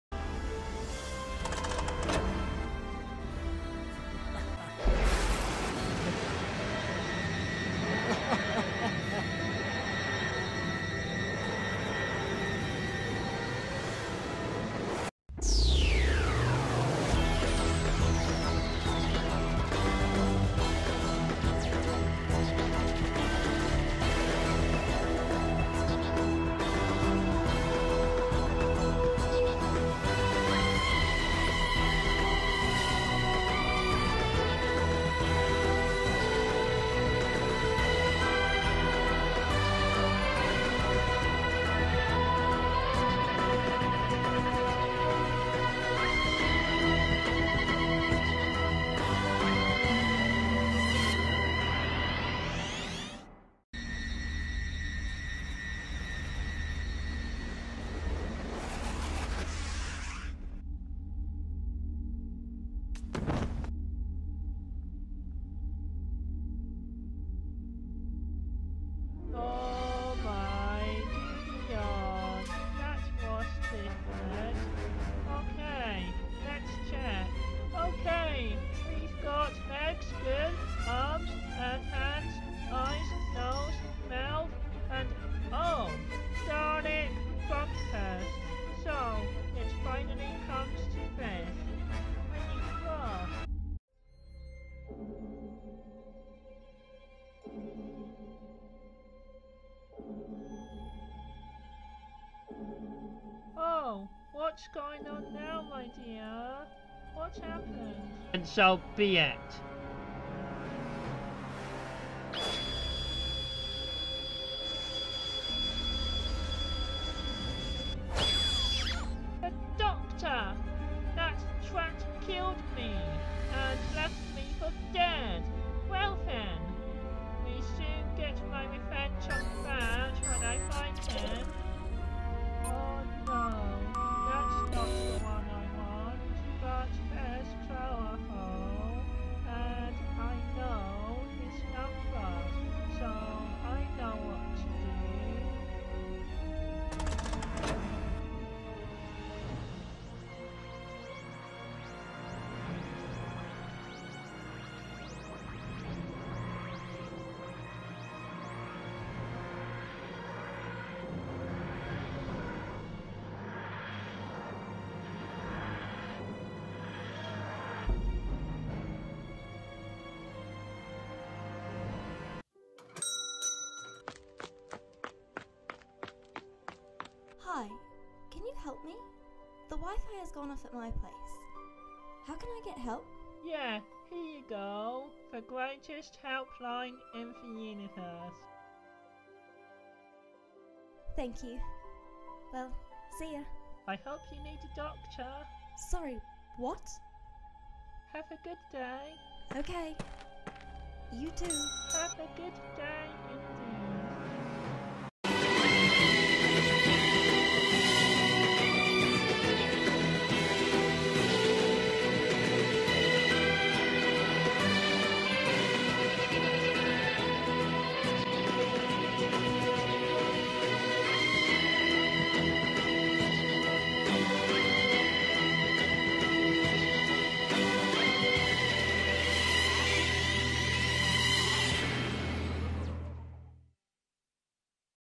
Doctor Who FA Minisode Missy
This is a aftermath from the series 5 finale of the master falls where the Master gets killed by the Doctor then he went back into his Tardis and regenerates into Missy as she remembers how she died and plans for her revenge So Enjoy and Subscribe For More? :)